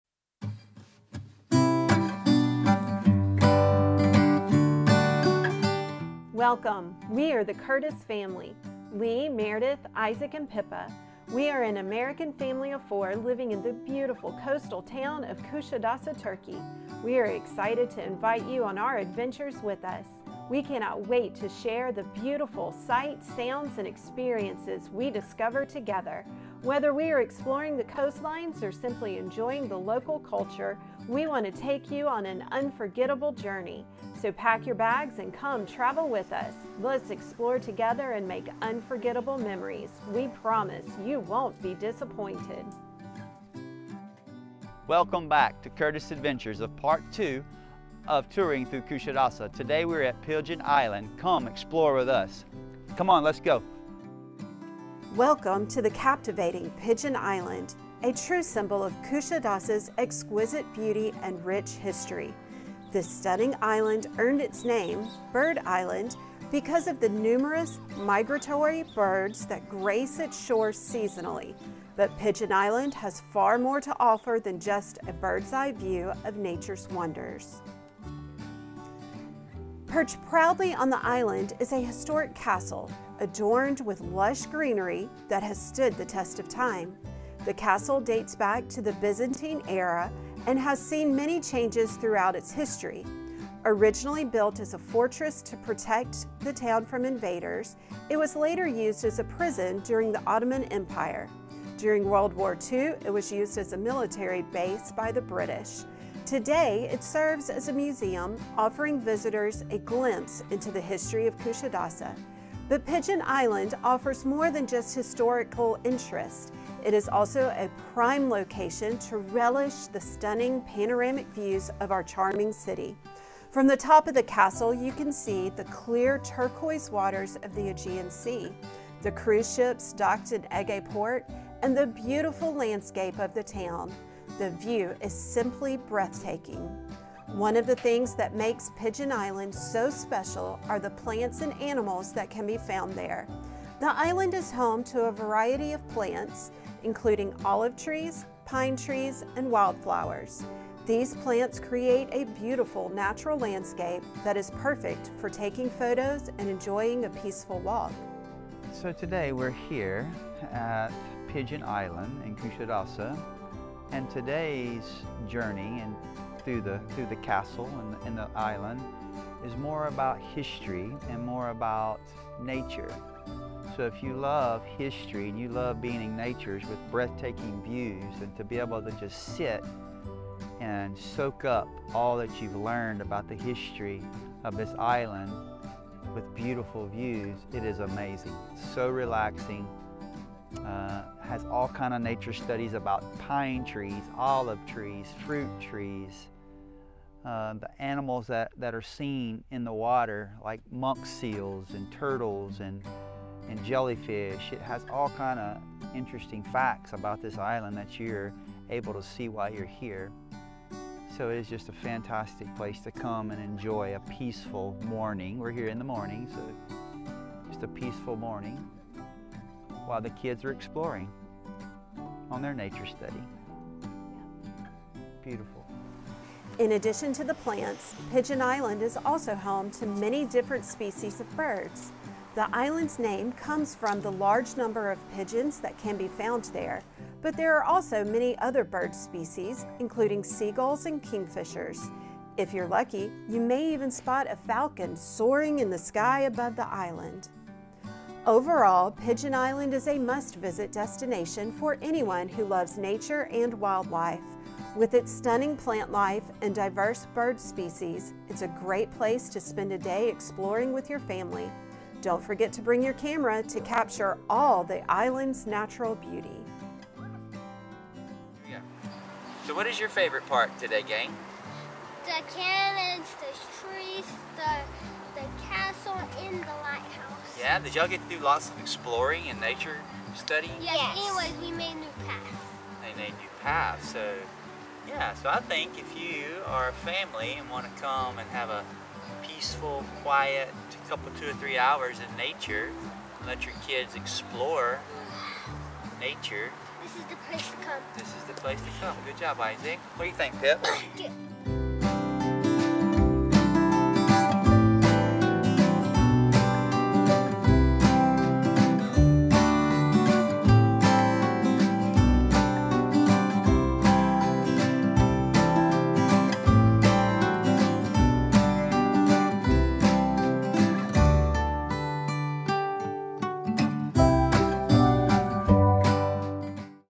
Get Lost In History At Pigeon Island Castle In Kusadasi, Turkey!
Welcome to our latest adventure here in Kusadasi, Turkey! Today, we're going to take you to the stunning Pigeon Island Castle, a true gem of the Aegean coast.

As we approach the castle, you'll be amazed by the breathtaking views of the turquoise sea and the lush greenery that surrounds it. The castle itself is a magnificent sight to behold, with its ancient walls and towers that stand tall against the blue sky.

As we explore the castle, we'll take you back in time to the days of the Ottoman Empire, when this castle was an important strategic point for the defense of the coast. We'll show you the impressive cannons that were used to protect the castle from invaders and the fascinating underground tunnels that were used for smuggling goods.

But that's not all! Pigeon Island Castle is also a great spot for snorkeling and scuba diving. The crystal-clear waters around the island are home to a rich variety of marine life, including colorful fish, octopuses, and even sea turtles!
So come on, join us on this exciting adventure and discover the beauty and history of Pigeon Island Castle in Kusadasi, Turkey.

00:00 Introduction
00:42 Welcome
01:05 How it was named
01:16 Castle History
01:53 Panoramic Views
02:17 Plants & Animals
02:39 Lee's thoughts
03:46 The Birds of The Island
04:08 Kids Exploring
04:28 Kids' Thoughts